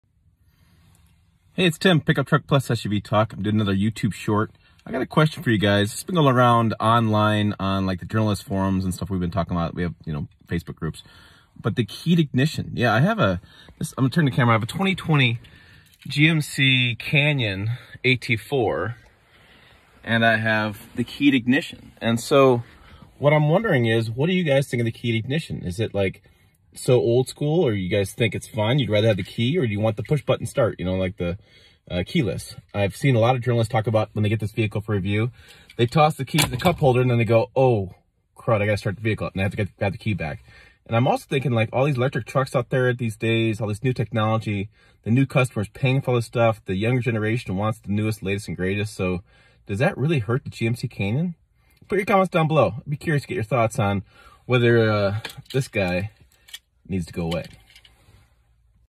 YouTube - are keyed ignitions in trucks just too old school?
In this YouTube we are asking you if keyed ignitions in trucks are too old school for new buyers.